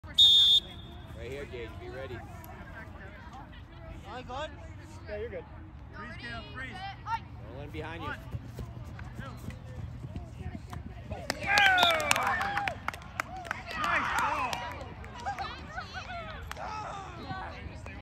8054 Silky Hogan-Cameraman gets late start Bucs Can't defend Xanders long bomb pass play! TD Score!
Bucs Clinch First Place!
Buccaneers Highlights, Spring League , Cawley Division Game 7, Hooksett Flag Football, 06/08/23 7:30 pm Donati Field 1,  Bucs 18 vs 14 Chiefs